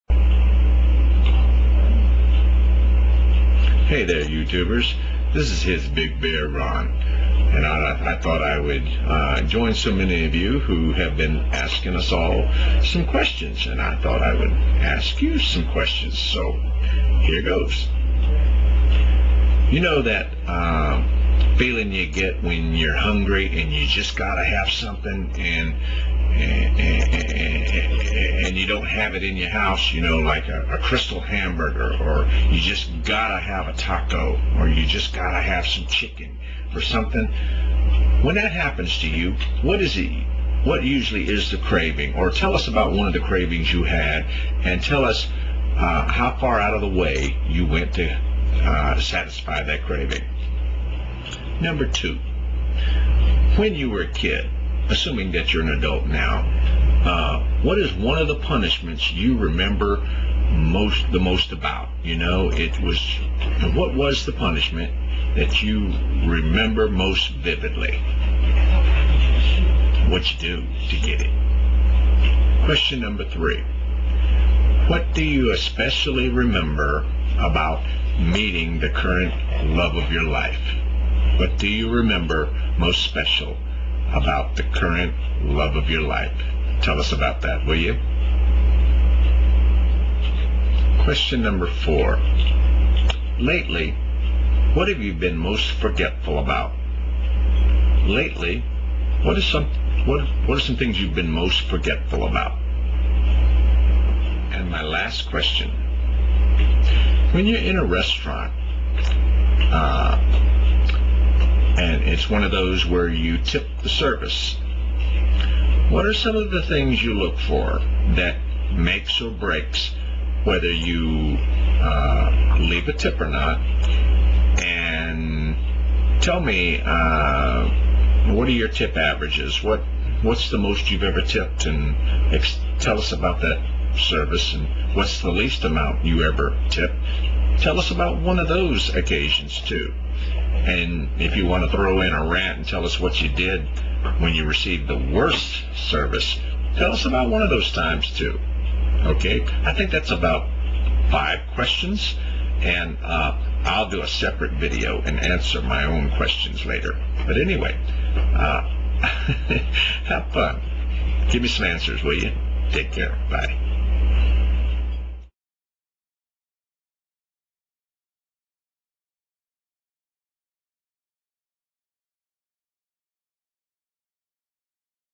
5 Questions for YouTubers
Listen, and you'll hear questions about cravings, punishments, love, forgetfulness, and tipping.  Have fun!

1.  When you've had craving fits, what have they been for, and how far out of your way did you go to satisfy it?

2.  What's one childhood punishment that you remember most vividly? (What did you do?)

3.  What special memory do you have about meeting the current love of your life?

4.  Lately, what have you been most forgetful about?

5.  When at restaurants where you tip the service, what have been the litmus tests (the making and breaking points) for the tips you've left on the table?  What's the most you've tipped?  The least?   Rant about a very bad service situation that ticked you off.